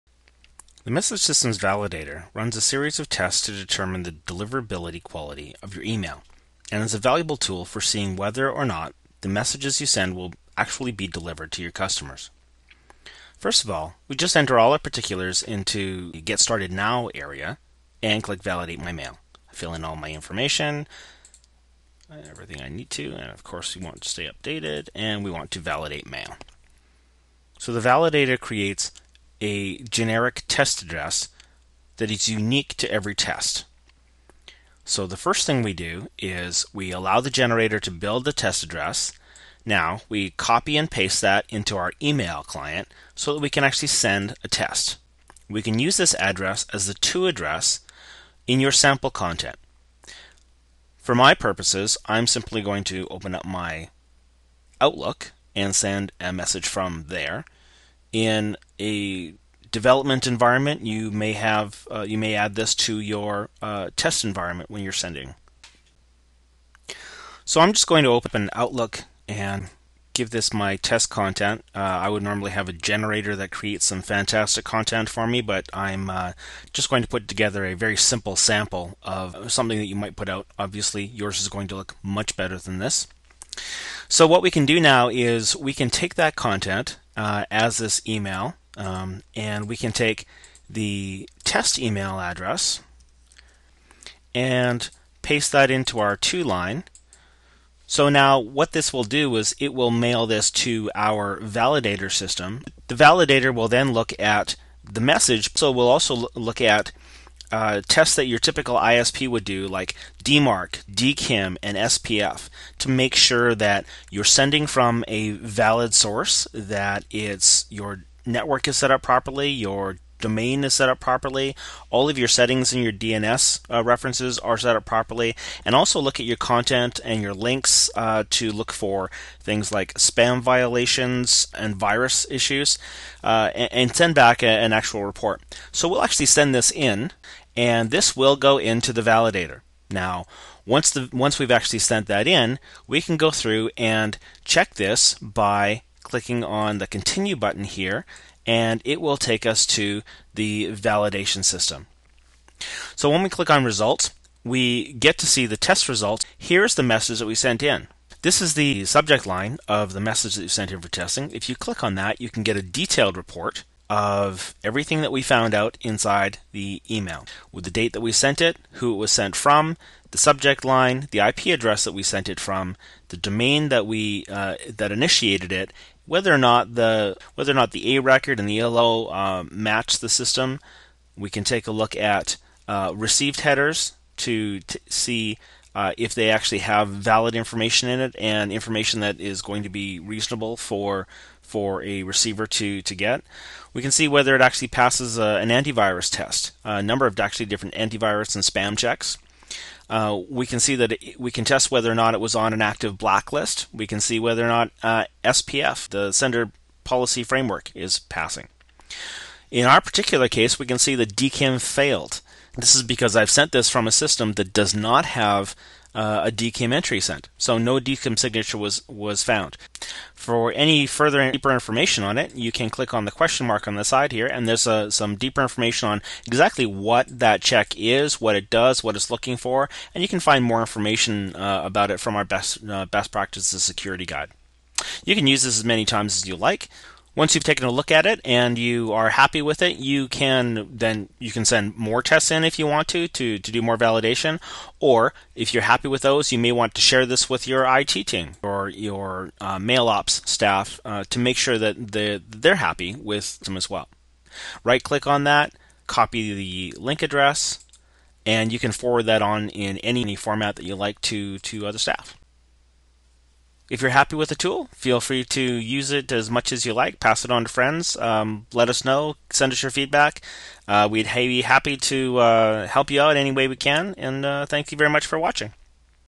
The Validator - A Free Email Authentication Tool
An all-in-one DKIM test, SPF checker and DMARC verification app, the Validator Tool is the ultimate test for email authentication and a direct route to ISP acceptance.

Want to know if your email is going to pass all the tests that Internet Service Providers will subject it to?